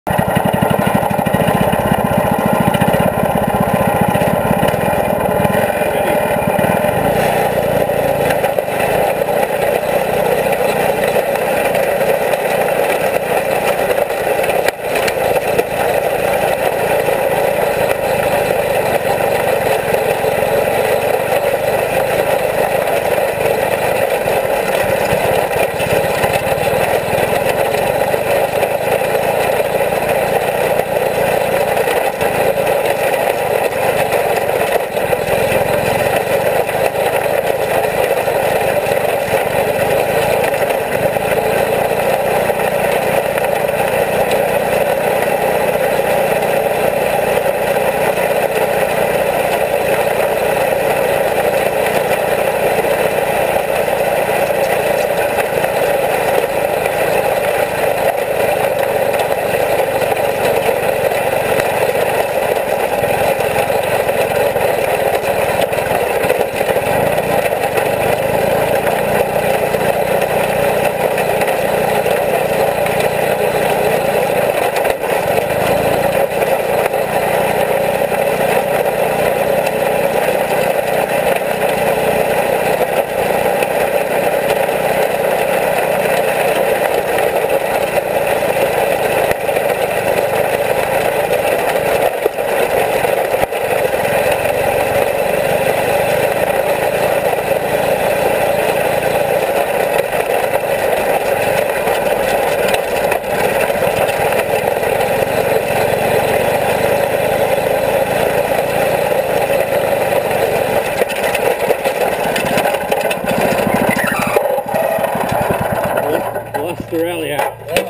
Black Valley Go-Karting 2013 Aldis "Setting the Time to Beat" with Titles!
Go-Kart time trials at Aldis & Cheryl's 25th wedding anniversary, AKA the 1st Annual Black Valley Rally! Aldis Trial 2 "Setting the Time to Beat". This trial contains track landmark titles!